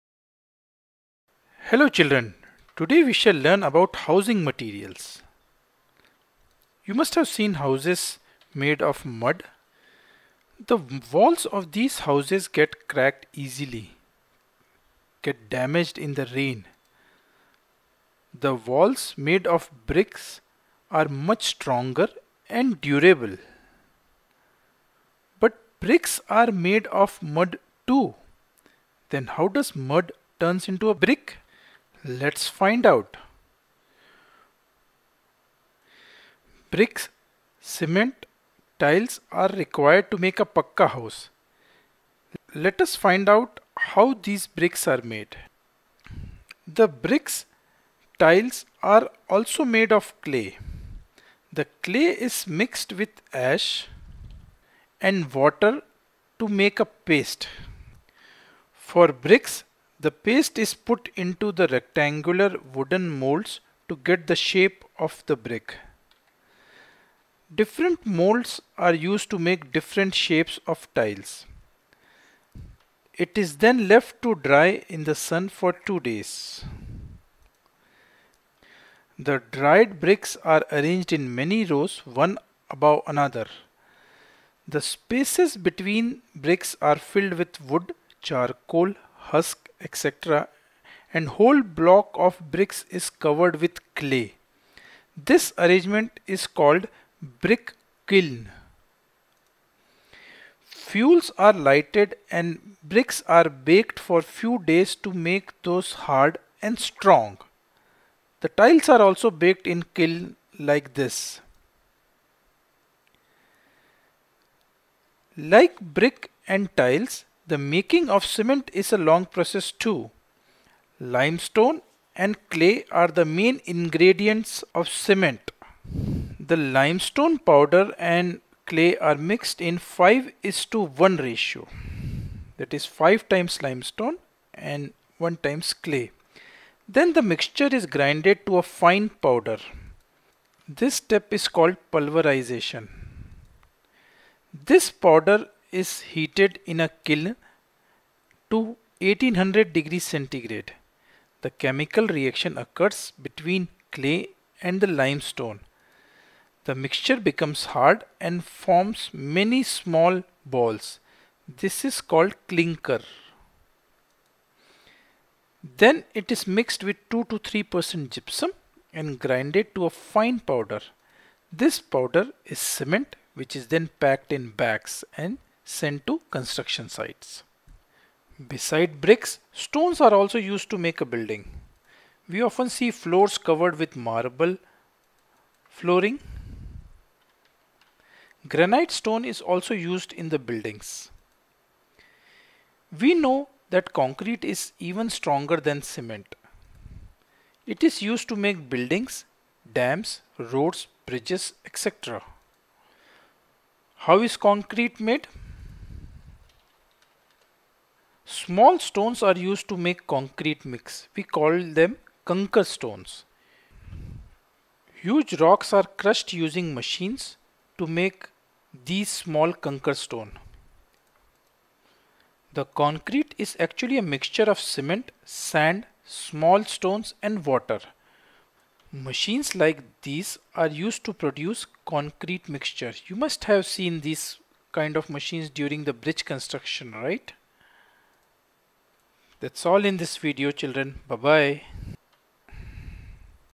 Science – Housing construction - different material – English
This video talks about importance of house and discusses about different housing materials.

Team BodhaGuru